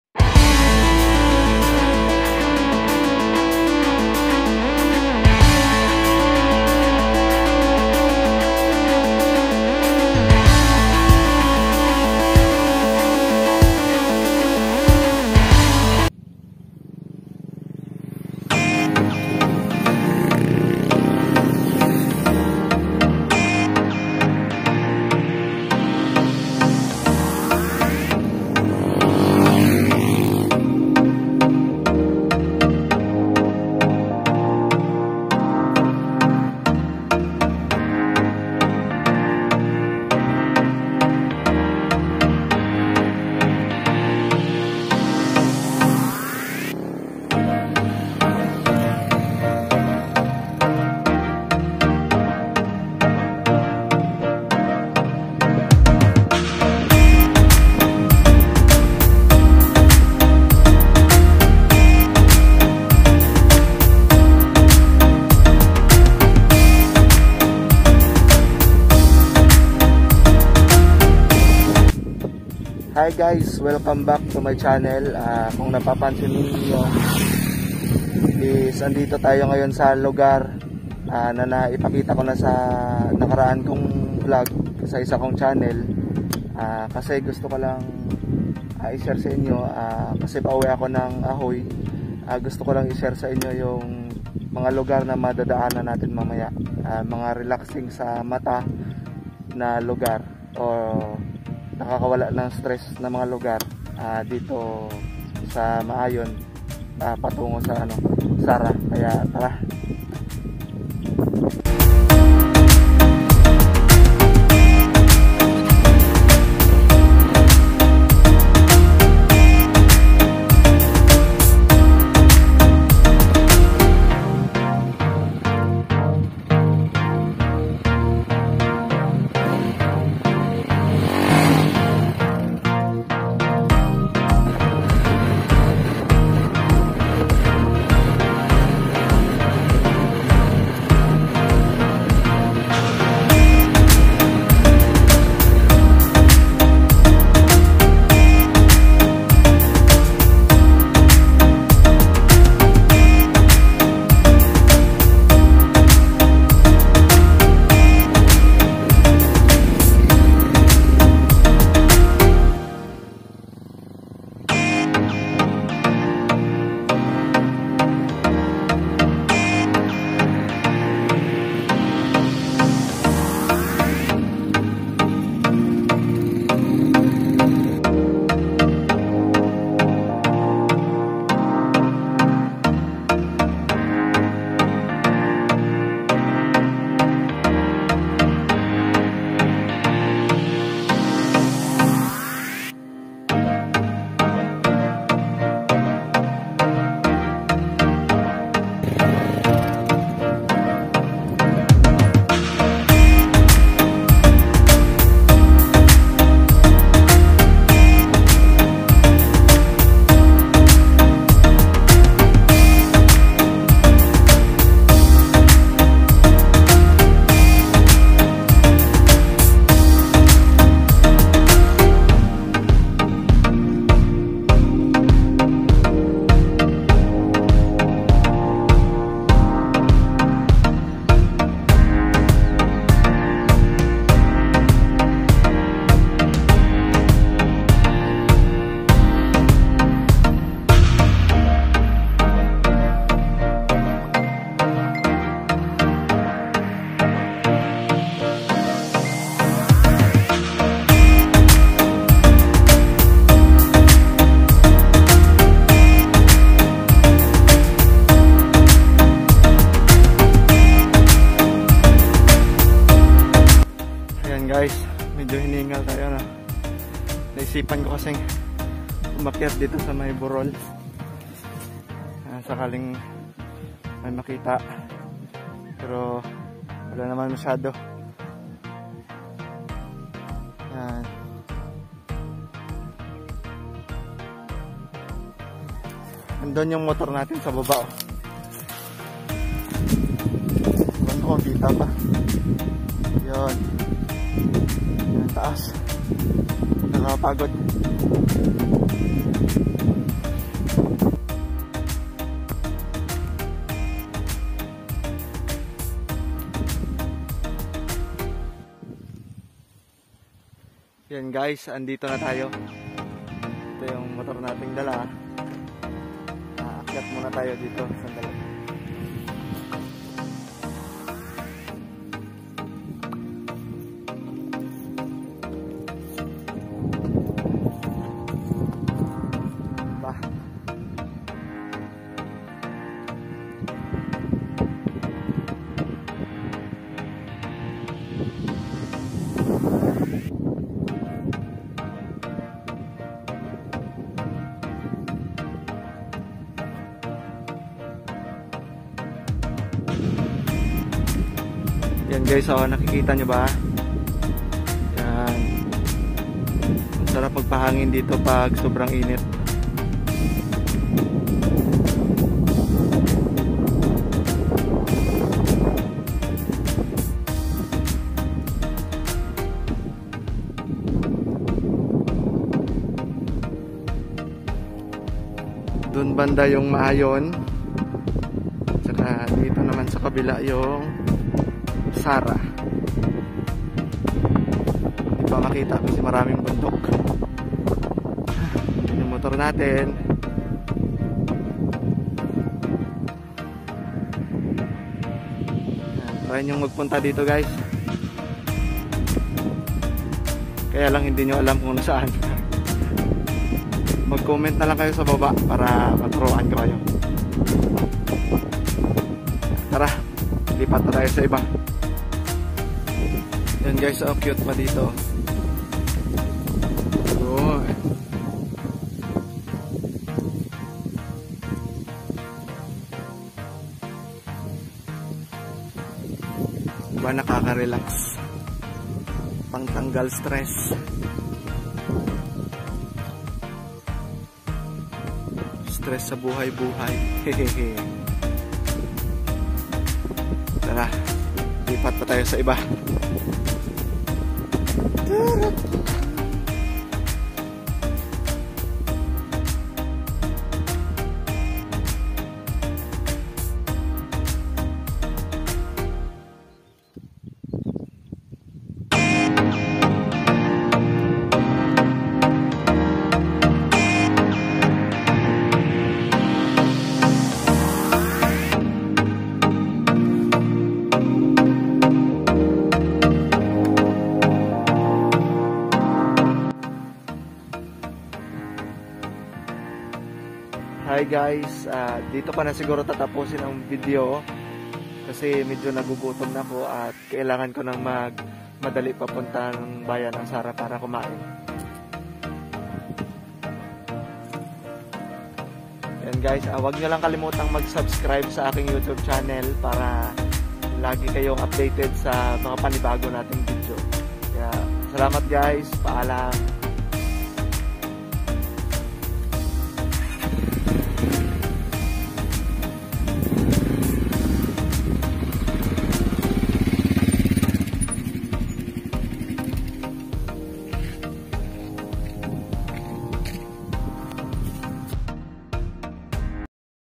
MAAYON, CAPIZ to SARA, ILOILO
share ko lang guiz ang hindi pa masyadong pamilyar na daan mula maayon, Capuz to Sara, Iloilo.